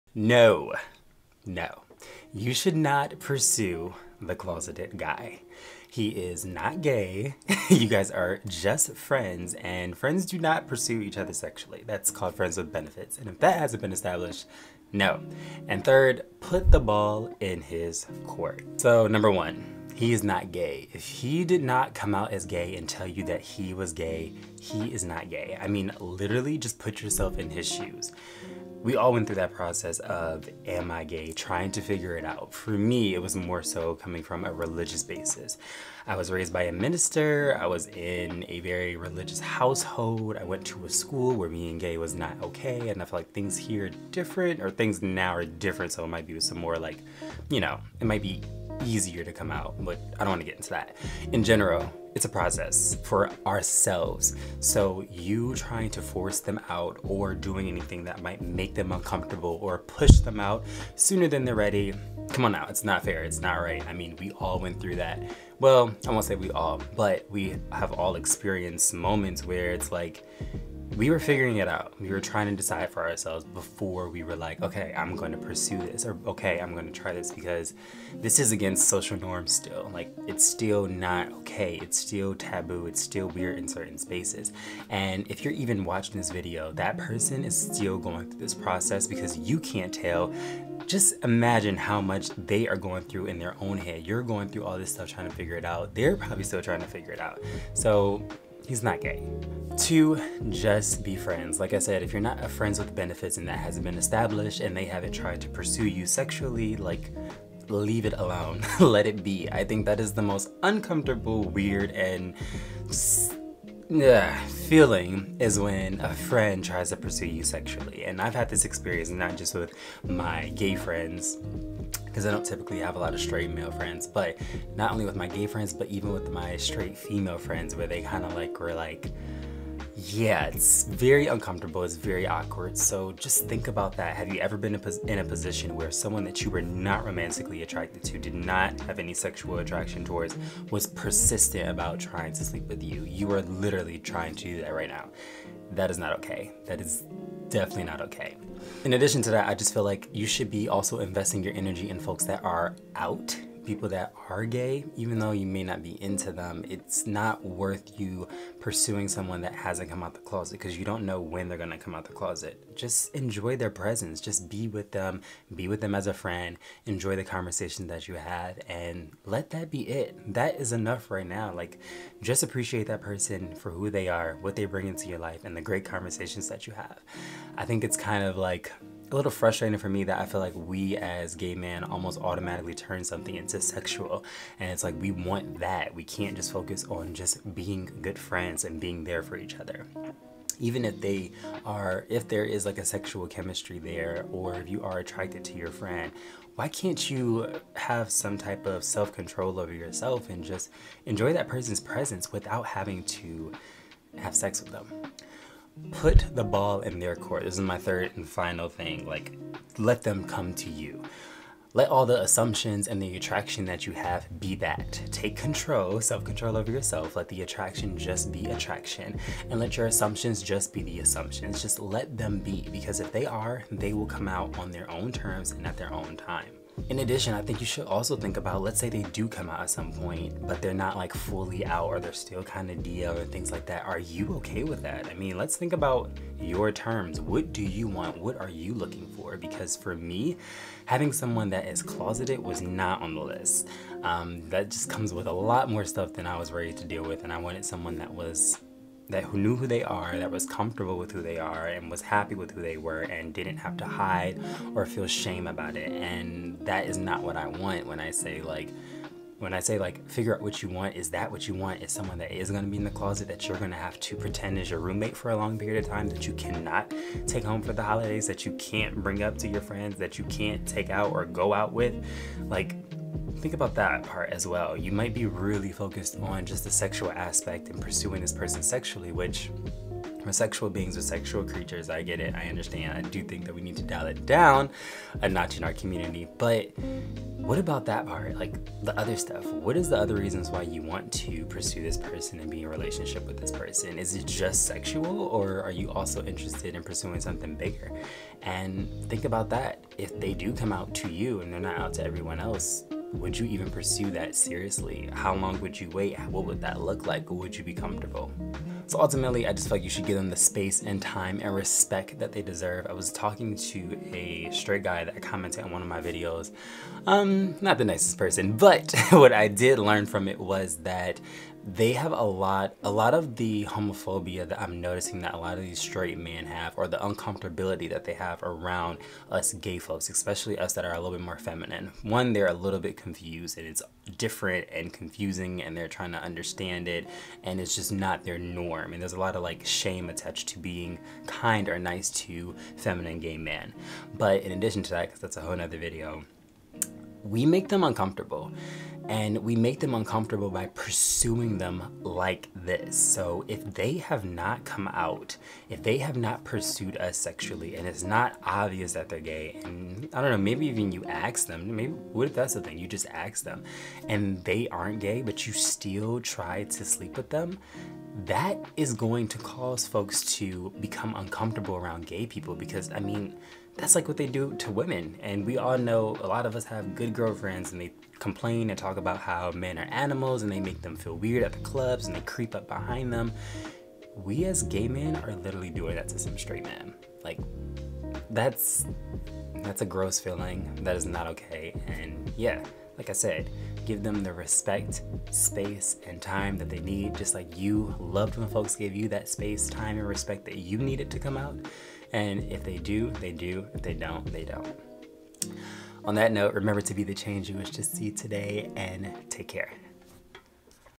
pursuing a closeted guy | a gay, black boy, narrative
no! do not pursue the closeted guy. my intention is not to present the obvious reasons, but to shift your perspective as to how it feels for them to be pursued when not desiring to come out of the closet..